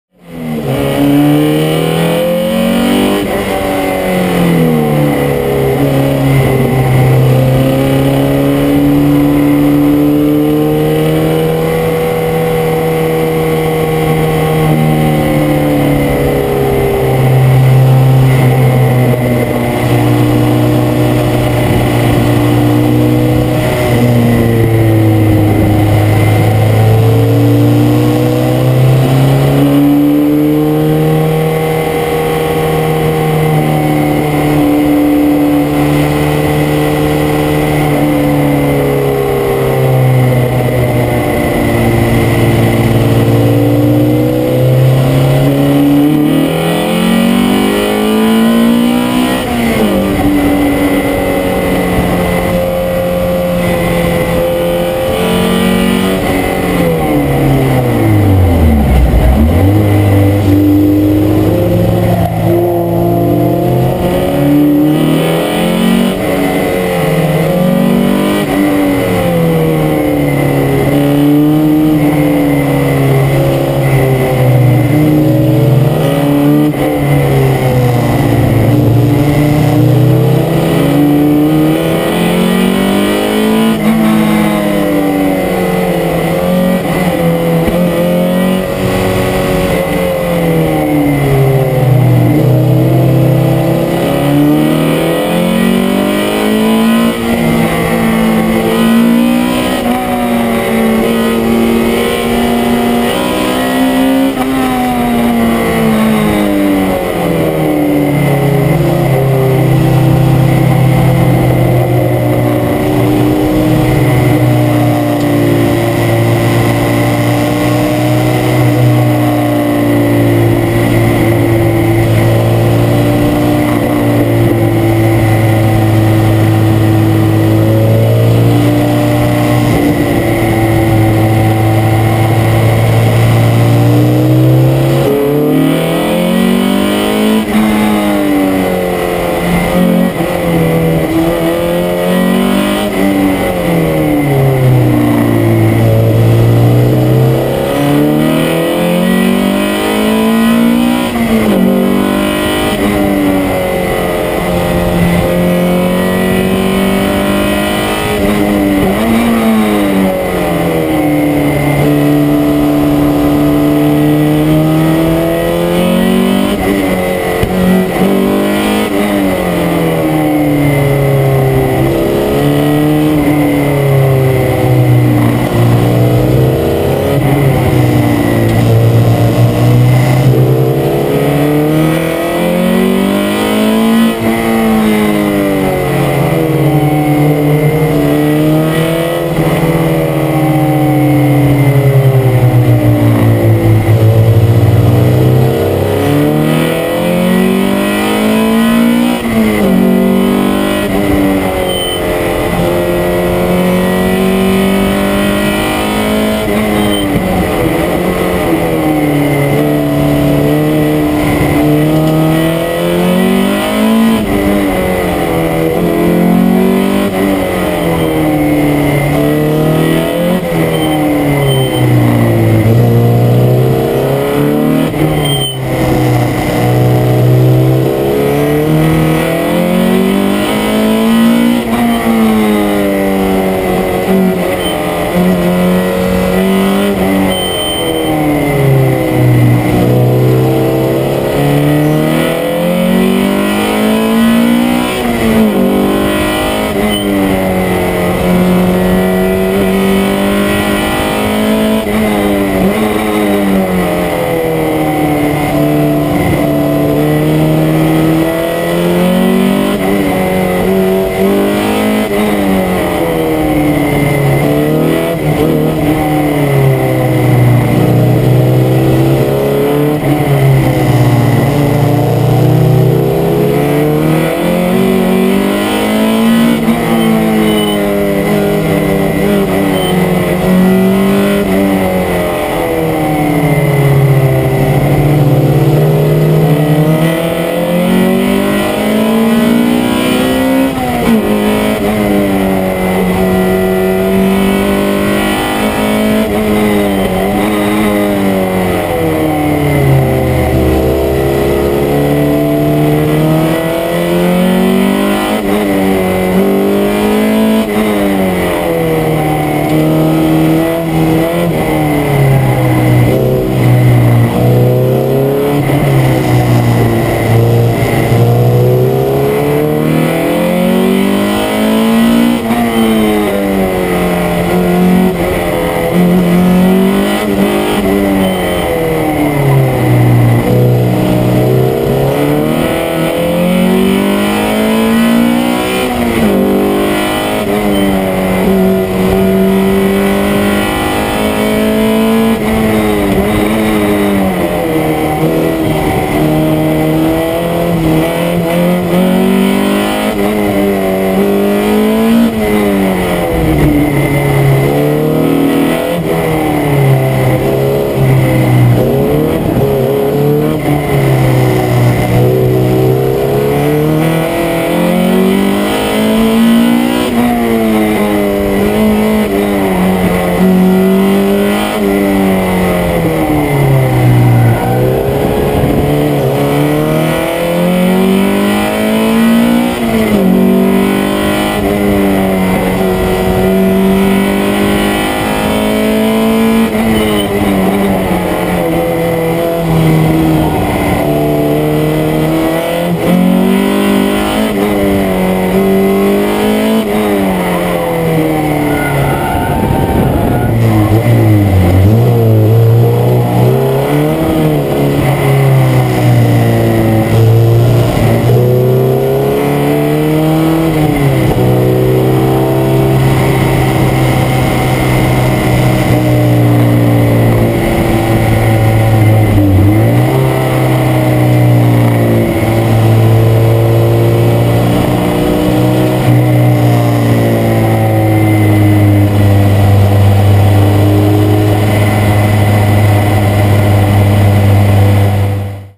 Trackday Lotus Elise K20
Trackday at La Chatre, France, 27/04/2105,

- First session of the day, first time with my new engine, new tires, new dampers, etc etc

- Première session de la journée, première fois avec mon nouveau moteur, et toute la réfection de la voiture, gros gros potentiel, reste à roder le "pilote", mais le plaisir y est !! :D